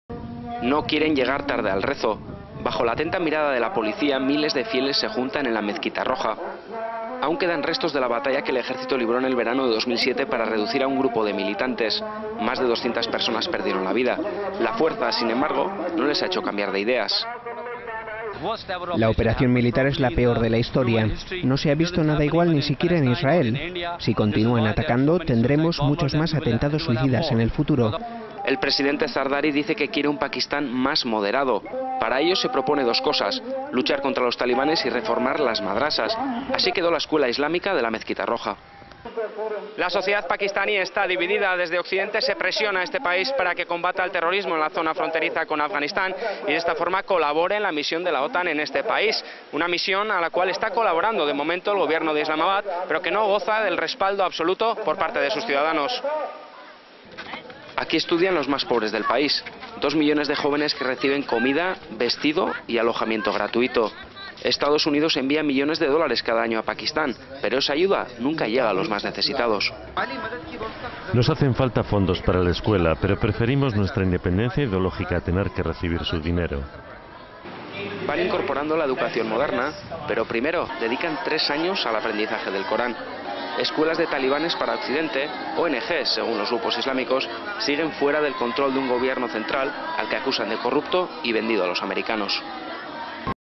PAKISTAN. Madrasas contra el Gobierno (06JUL09)
Estados unidos prosigue su guerra contra el terrorismo en Afganistan para la que necesita contar con el apoyo del vecino paquistani. Pero,  ¿q opinan los paquistaníes de esta guerra? no toda la sociedad la apoya, ni mucho menos. Los sectores más islamistas, que son muy importantes en el país,  acusan a si gobierno de tratar a los musulmanes incluso, peor que el estado de Israel.